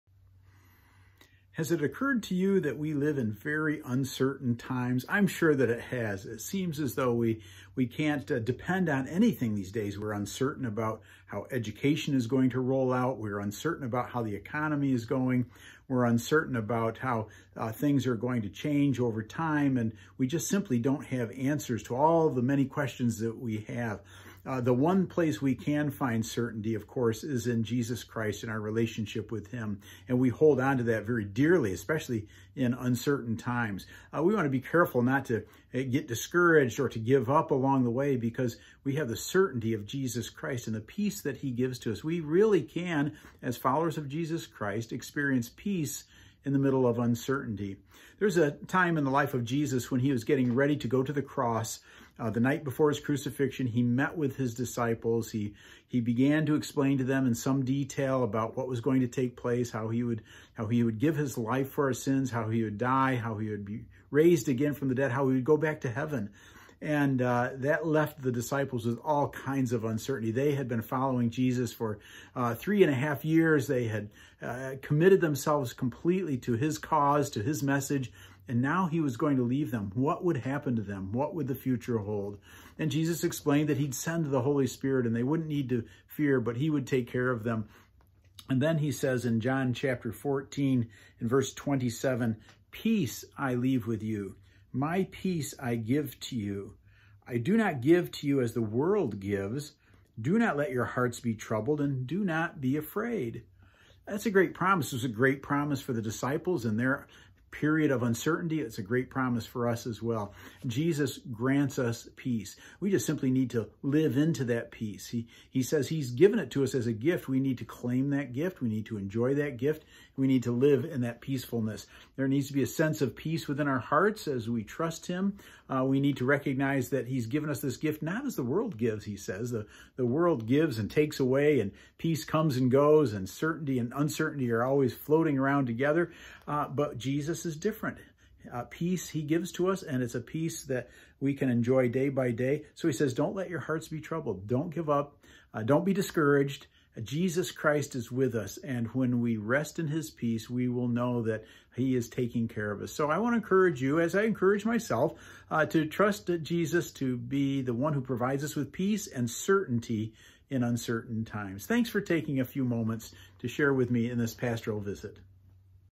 Dana's Weekly Visit 8-28-20
Dear Pittsford Community Church Family,

I hope everyone is enjoying these final days of summer.  With fall plans and school re-openings just around the corner, everyone seems to have a full plate.  But God is our guide.  Keep your head up and your heart in tune.  Fall is a great time to start or join a small group (in-person or virtually).  Consider participating in Pittsford Bible Institute’s course on Bible interpretation, joining the Whetstone Group for leadership enrichment, or other Bible study and fellowship groups.  And while I’m thinking of it, please pray for the leadership team as we seek God’s vision for PCC in this season of opportunity.

Pastor Dana

P.S.— Take a moment to listen to this week’s Pastoral Visit video about peace in uncertainty.